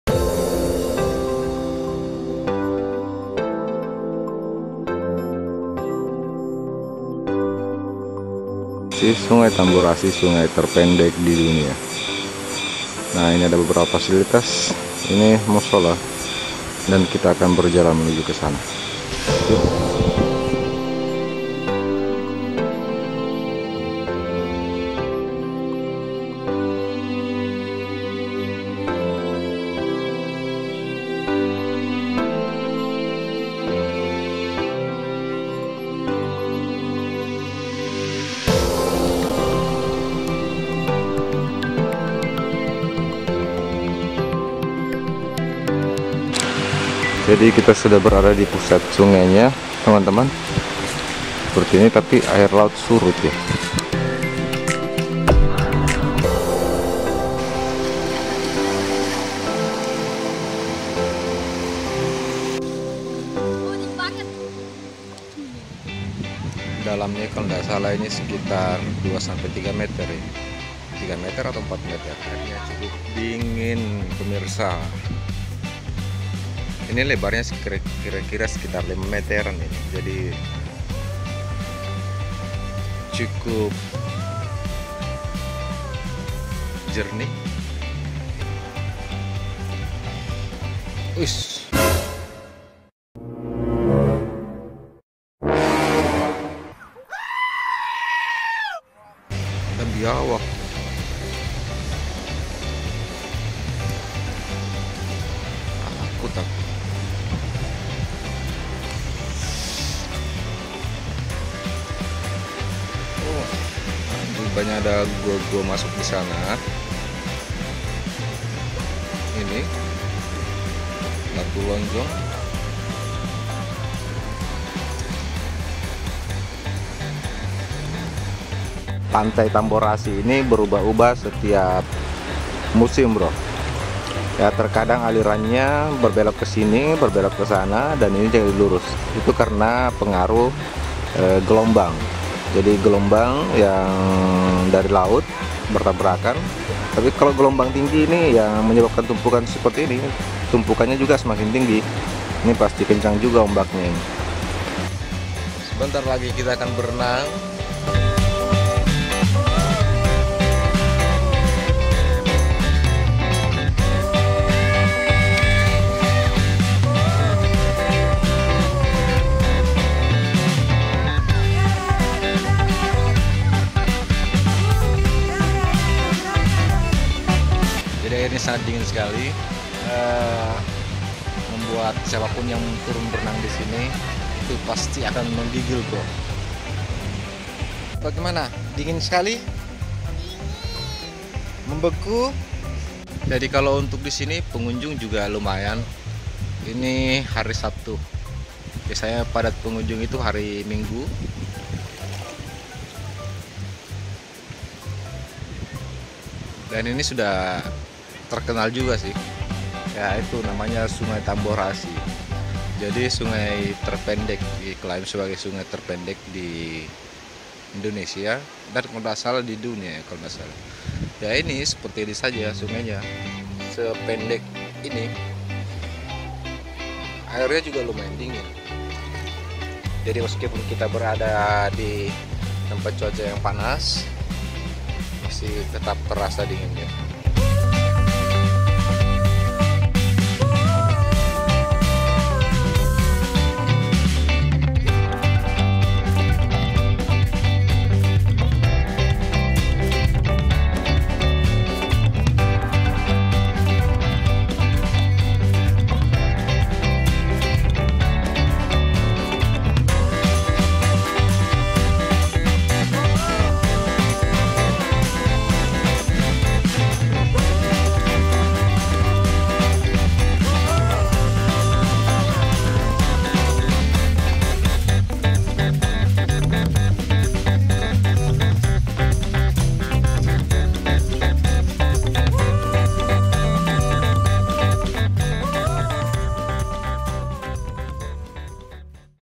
SUNGAI TERPENDEK DI DUNIA YANG ADA DI KOLAKA | TAMBORASI 💦🏊‍♂️
Sungai Tamborasi terletak di Desa Tamborasi Kecamatan Iwoimendaa Kabupaten Kolaka Provinsi Sulawesi Tenggara.